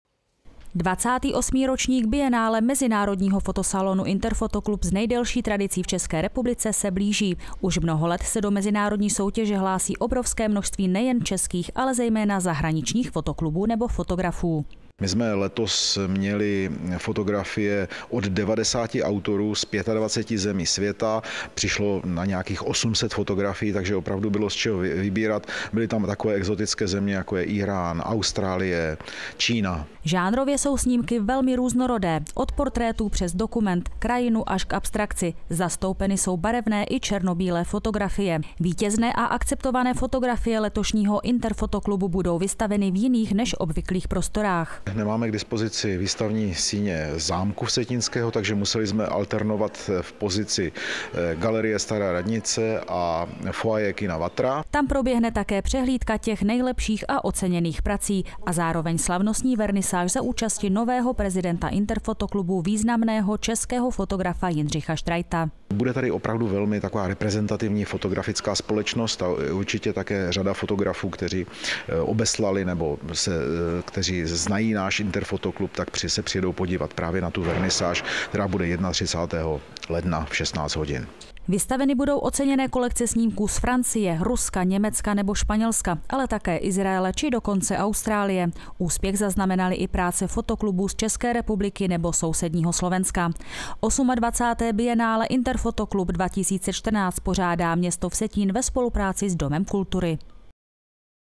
INTERFOTOKLUB Vsetín 2014 představí oceněné snímky světových fotografů
Snímky z Francie, Ruska, Německa nebo Španělska, ale také Izraele či dokonce Austrálie nebo Číny. Až z takto vzdálených zemí přišly fotografie do 28. ročníku fotografické soutěže INTERFOTOKLUB 2014. Oceněné a akceptované snímky budou k vidění už poslední lednový den, kdy v koně vatra proběhne slavnostní vernisáž.